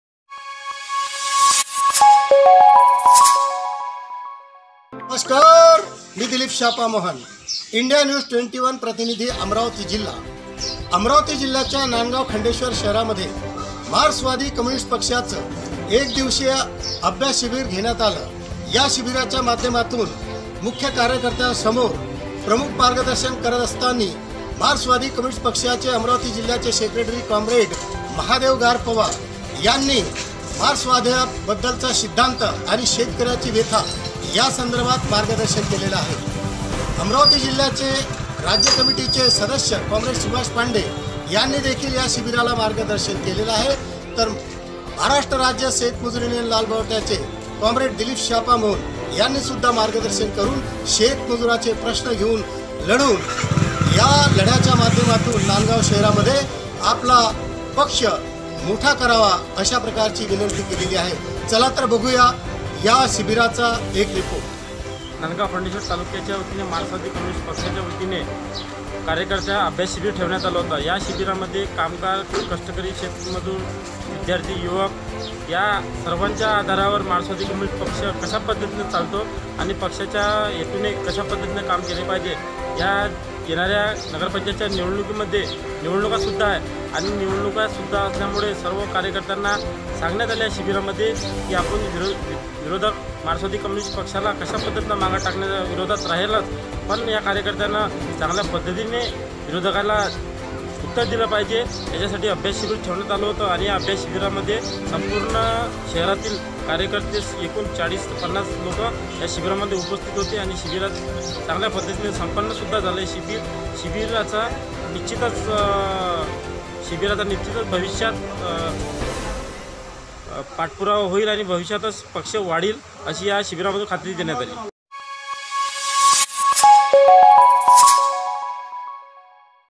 अमरावती; नांदगांव खंडेश्वर येथे मार्क्सवादी कम्युनिस्ट पक्षाच्या कार्यकर्यांचे अभ्यास शिबीर..
इंडिया News21